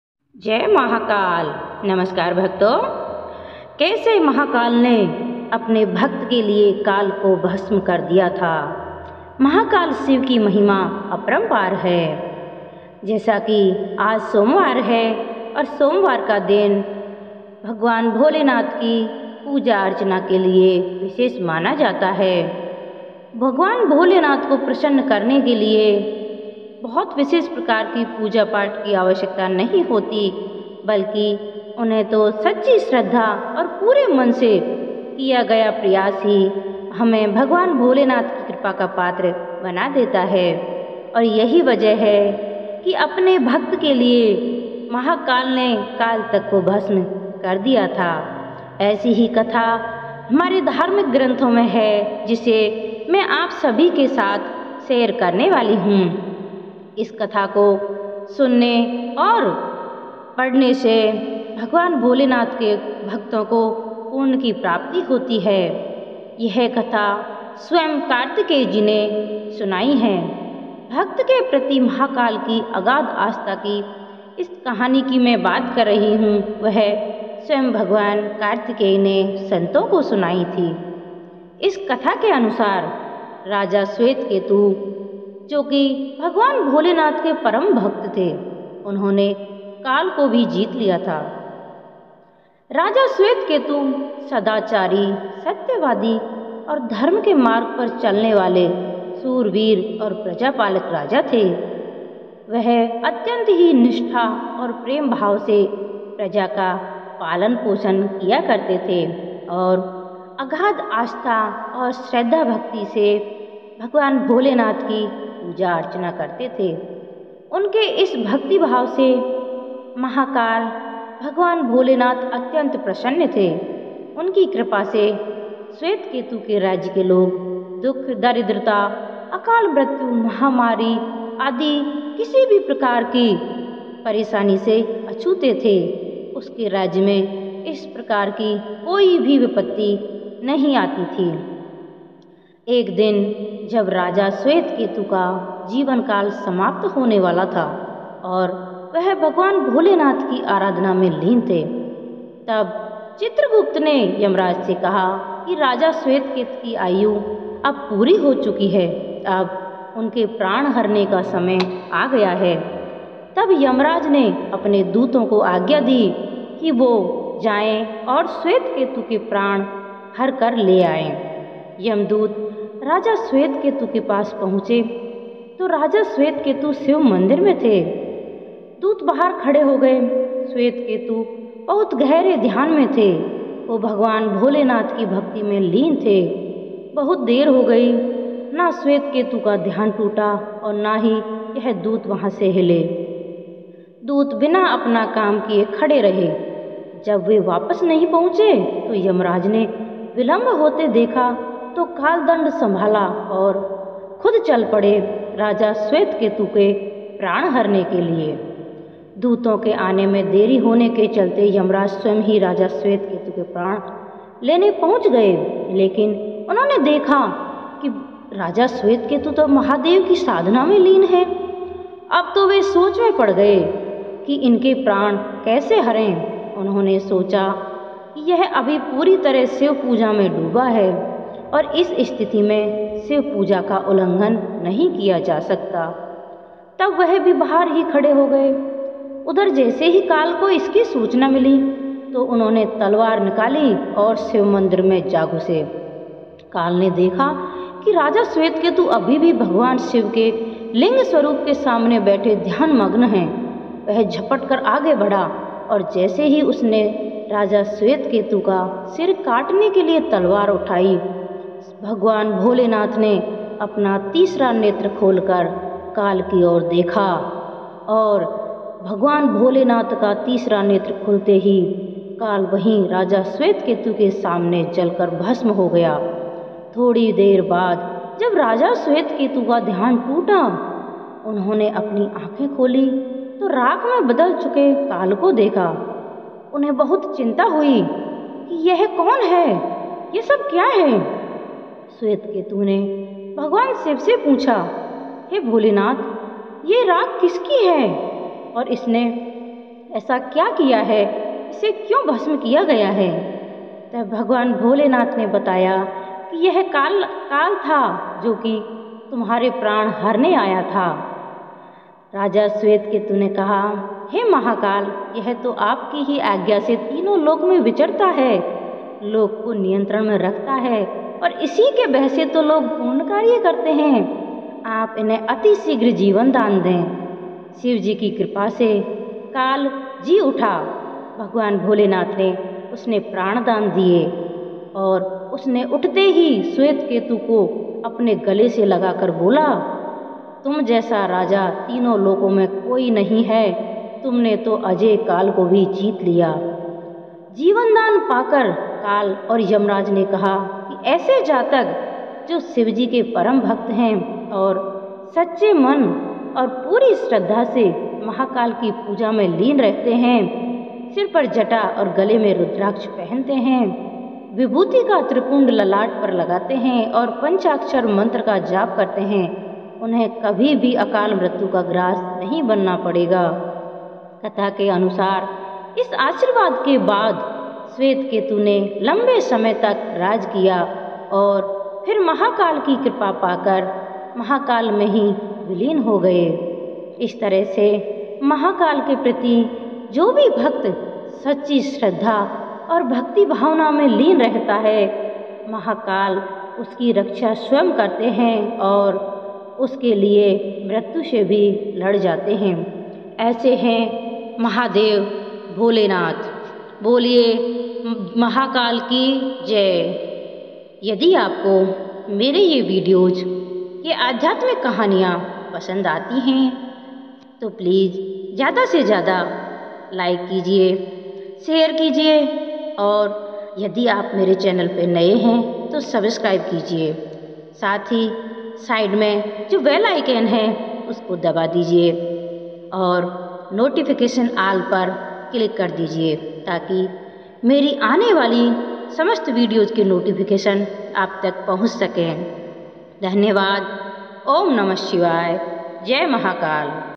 भक्त के प्राण बचाने के लिए भोलेनाथ ने भस्म कर दिया काल को/भक्ति की शक्ति/मनोकामना पूर्ति/ॐ
सोमवार व्रत कथा
motivational video
spiritual videos
religious videos